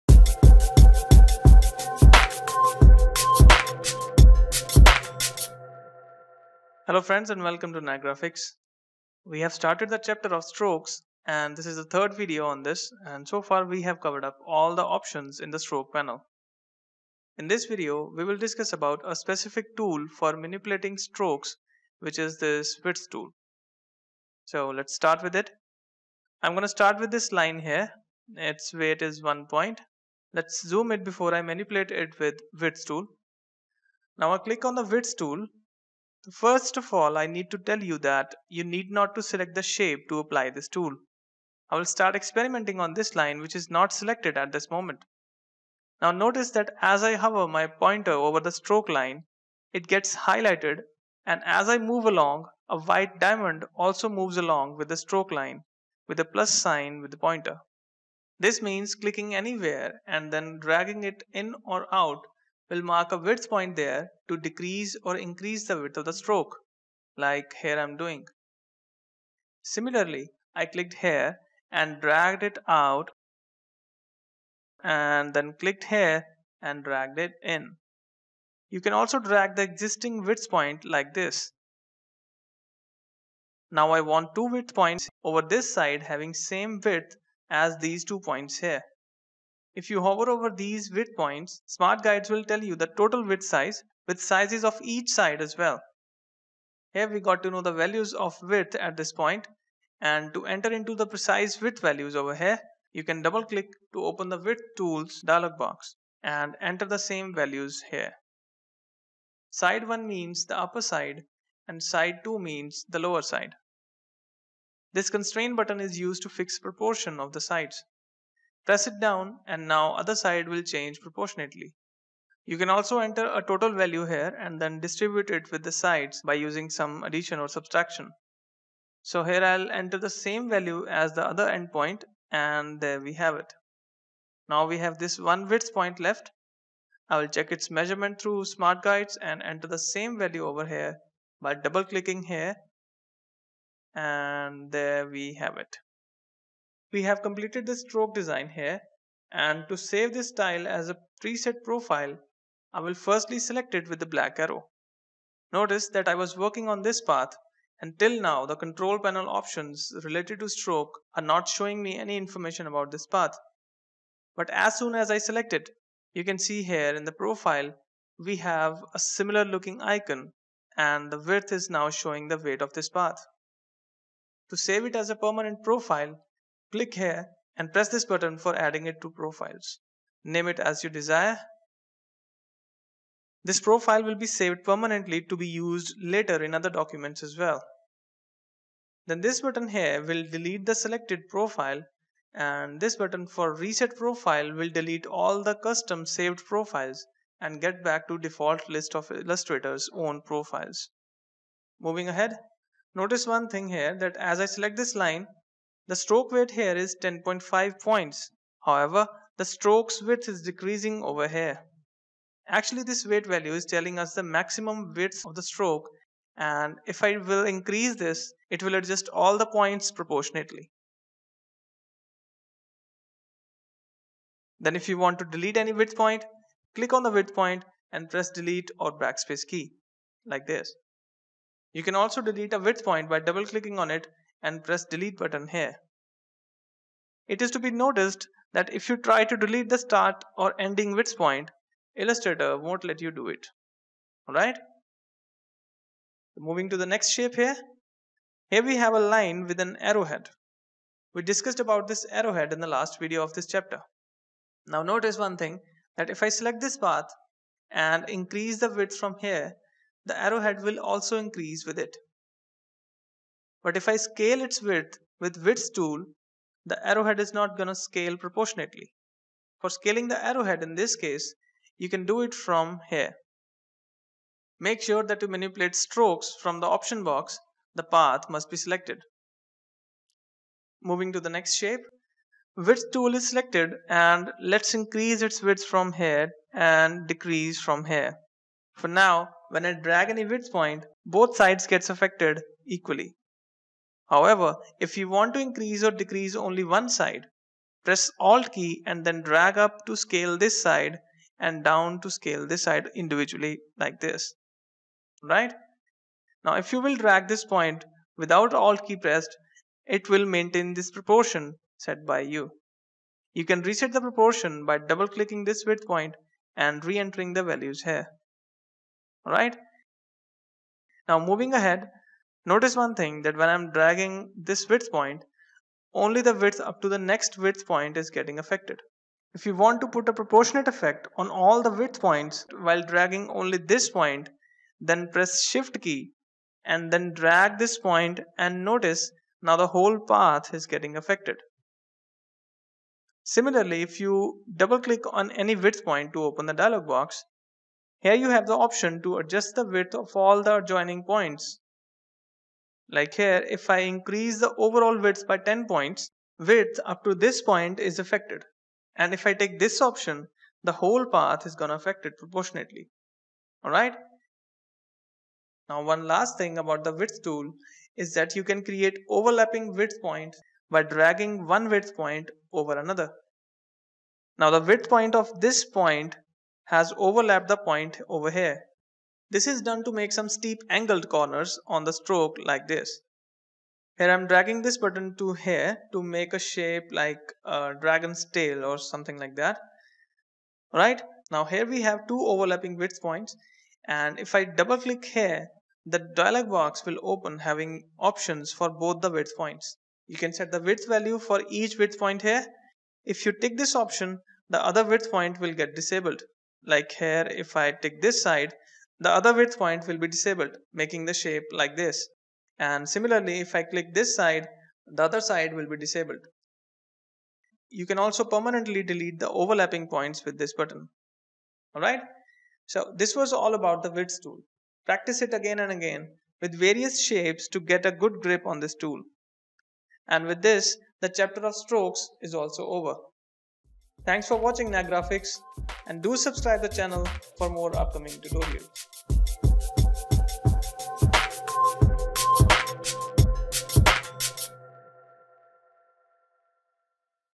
Adobe Illustrator CC Tutorial | How to use Width Tool C9-E3
In this tutorial, we will learn how to use the Width tool in Adobe Illustrator CC. All the related options are discussed with practical example.

Presented By:
Avinash